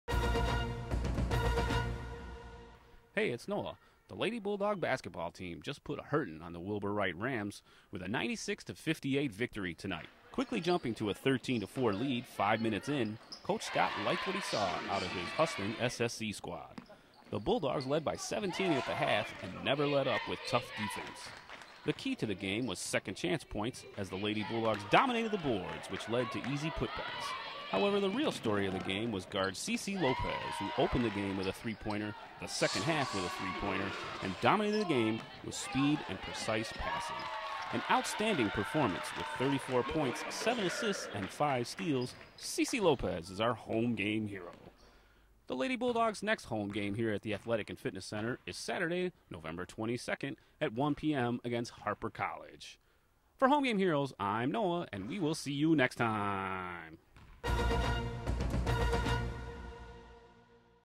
Women's Basketball vs. Wilbur Wright College - Home Game Heroes
The Lady Bulldogs were back in action taking on the Rams. Here's a recap of the game and who stood out on the court.